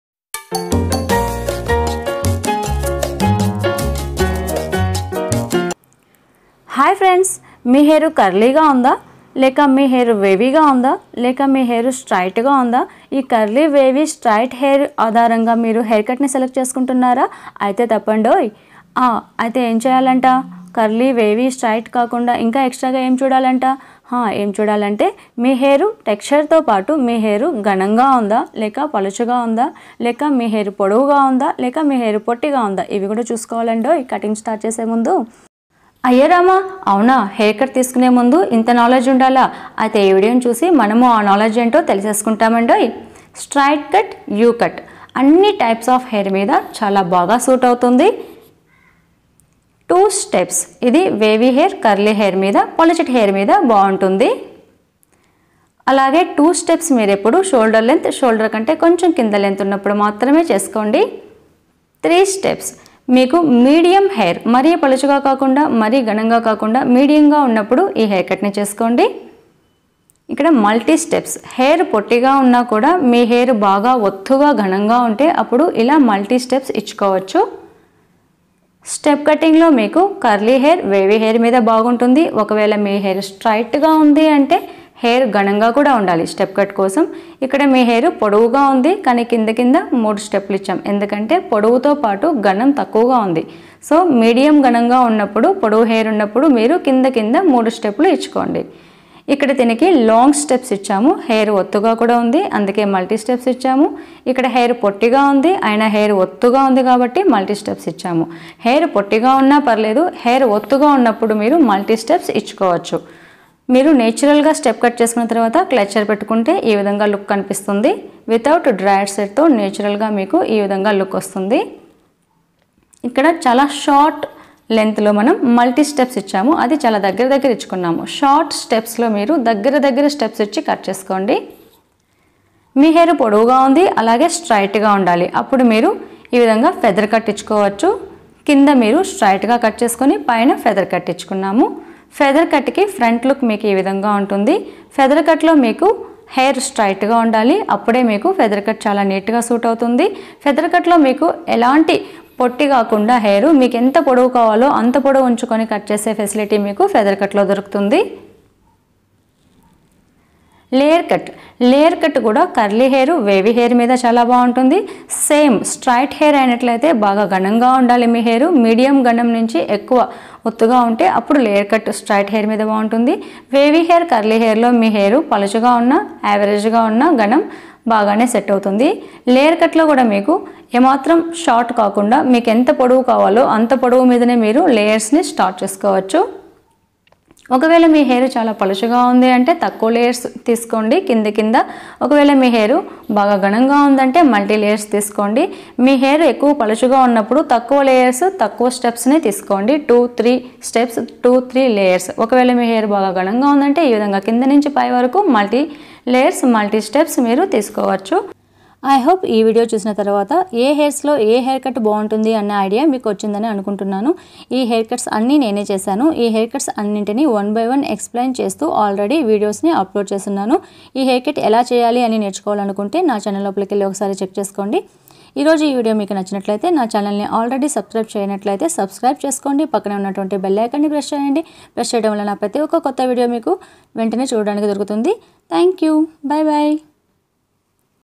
Which hair cut suits you?/ Types of haircuts with their names according to hair texture / Haircuts
Which hair cut suits you? | Hair cut tutorial / Types of haircuts according to hair texture / hairstyles / Haircuts

Hair cuts according to their hair texture